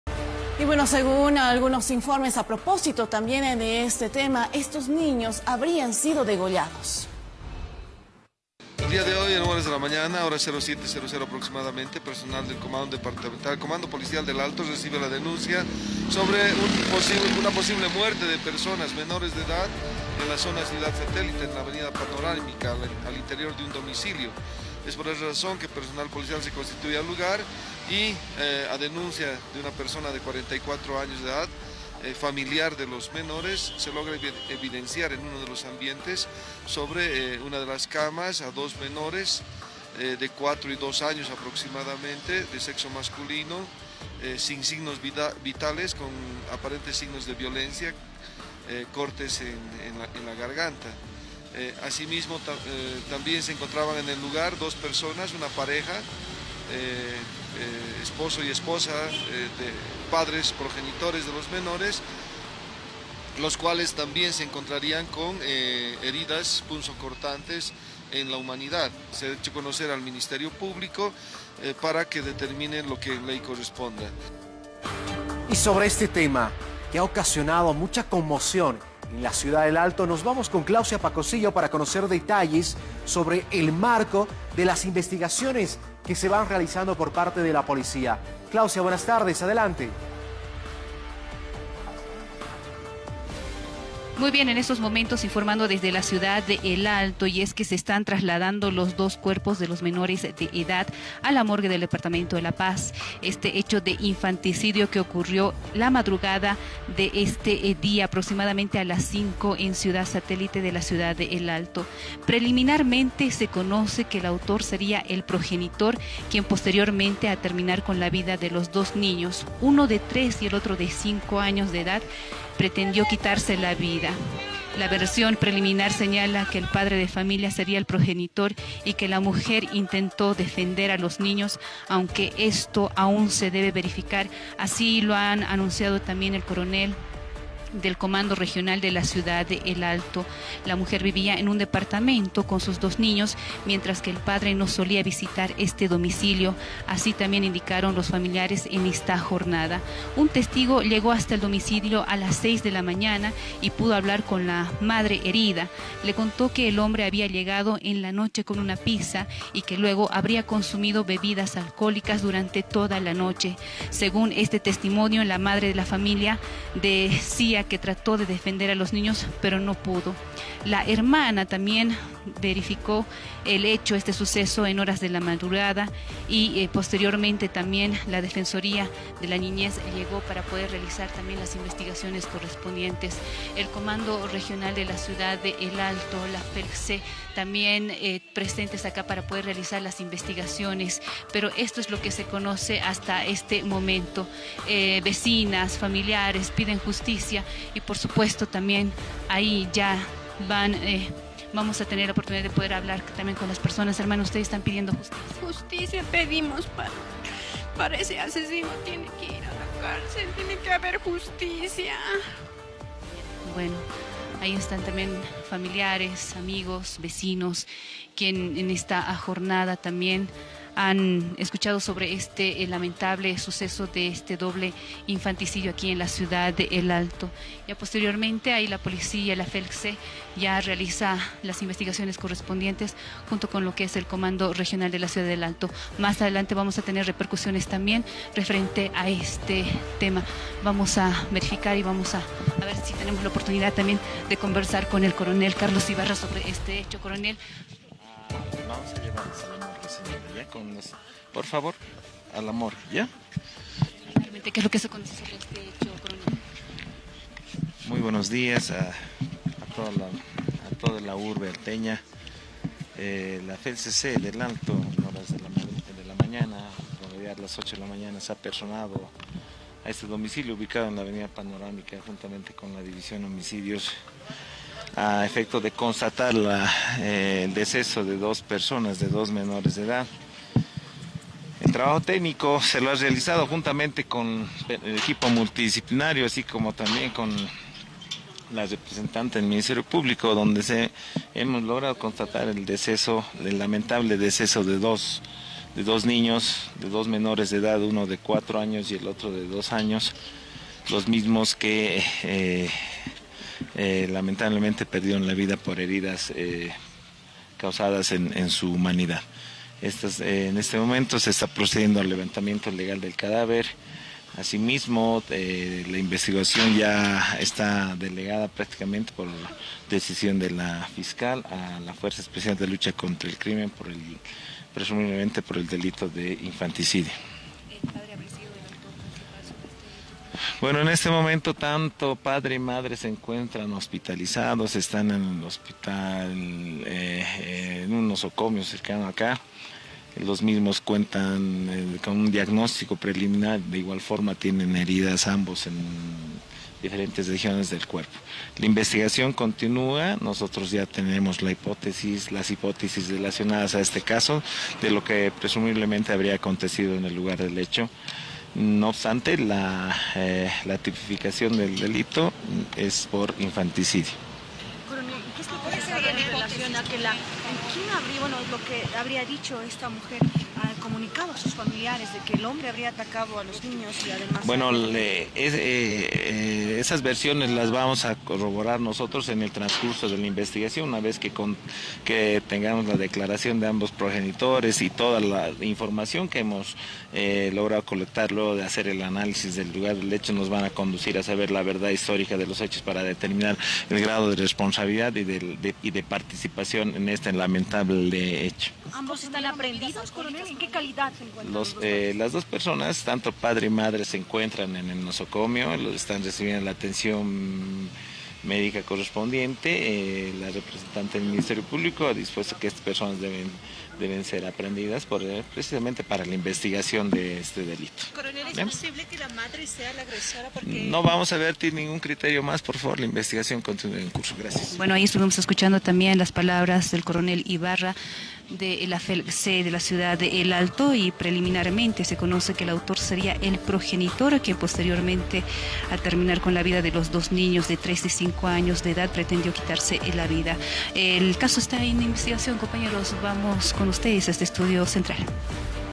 Doble infanticidio en El Alto; los menores habrían sido degollados
BTVInforma 🔴 | Los dos menores que fueron asesinados, presuntamente por su padre, en la ciudad de ElAlto, habrían sido degollados, La madre de los mismos también es investigada, informa el comandante departamental de la Policía, Édgar Cortez.
Los cuerpos son trasladados a la morgue de LaPaz.